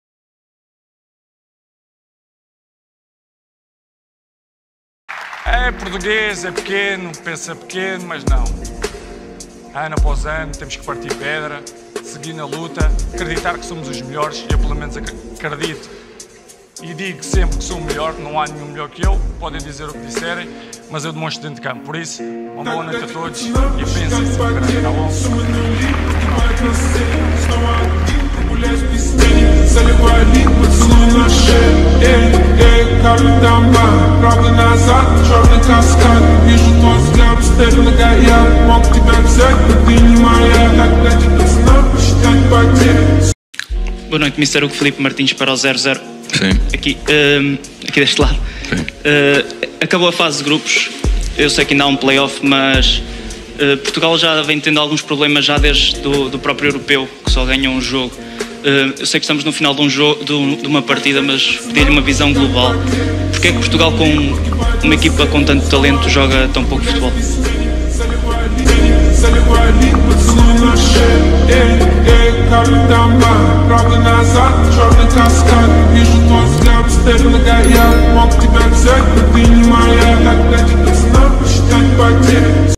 Siga estas Regras para se Tornar um Macho Alfa - Regras de Sigma Male [Cristiano Ronaldo] (1)
No vídeo de hoje vou começar uma nova série de memes baseados em Regras de Macho Alfa E Femea Alfa (Sigma Male/Sigma Female), em que consiste em tu seguires regras básicas de vários famosos que existem por aí espalhados no mundo inteiro. Começando hoje com Cristiano Ronaldo Aka Cr7 em que tens 3 regras simples: Pensar que és o melhor do mundo, seres o mais lindo e uma obrigatoriedade de seres Tuga e gritares Siiiiu.

*Tags Sujeitas a Alterações*

Cristiano Ronaldo
Fêmea
Macho
Sigma